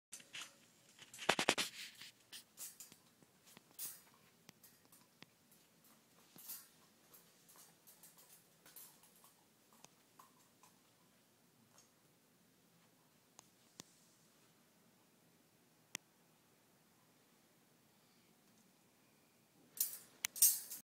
Toys are a waste of money
Why would you buy me toys when I love the container they are stored in.